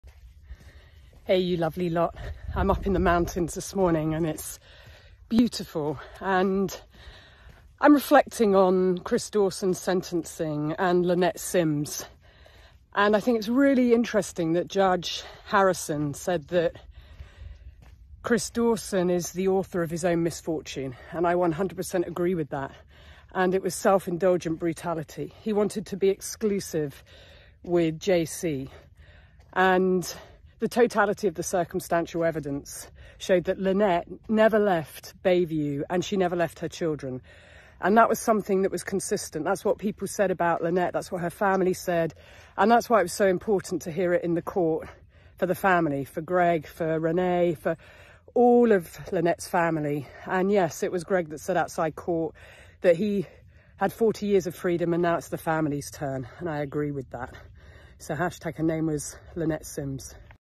Chris Dawson murdered Lynette because he wanted to be exclusive with JC, who was a minor at the time
Reflections: Chris Dawson murdered Lynette because he wanted to be exclusive with JC, who was a minor at the time. Lynette never left Bayview and never left her children, said Justice Harrison in his sentencing remarks. Lynnette’s family and friends said she was a loving, caring mother who never would have left her children. If only the police had listened to them